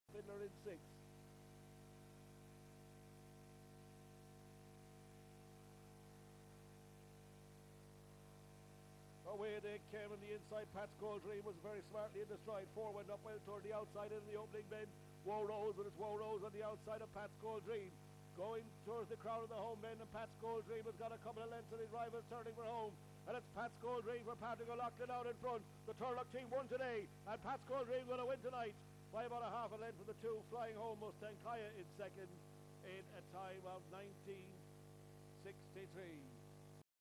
IGB - THE RACE TEXT ENTRY NUMBER 12/10/2019 Race 3 - Galway
Race: THE RACE TEXT ENTRY NUMBER
Track Name: Galway
Date: 12/10/2019
Hour: 20:22

Results

1st    1     PATS GOLD DREAM     5/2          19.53
2nd    2     MUSTANG KAYA        2/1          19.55
3rd    4     WOE ROSE            4/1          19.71
4th    3     FIFTY POUND LASS    5/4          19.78
5th    5     ROGERS AMBITION     5/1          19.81
6th    6     MENLO FIDDLER       3/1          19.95

|GLY_2019_10_12_03